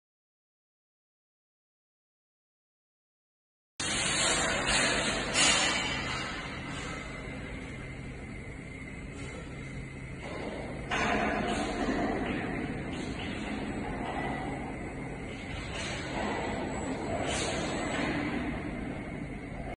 Welding - Seam Finding Touch Sensing
This video is a demonstration of a touch sensing program
System Specifications:
Yaskawa MH50-20II
Controller: DX200
Laser: Servo Robot
Welding power source: Fronius TPSi 500a

DPAC Welding Service is an automation company based in Queensland Australia.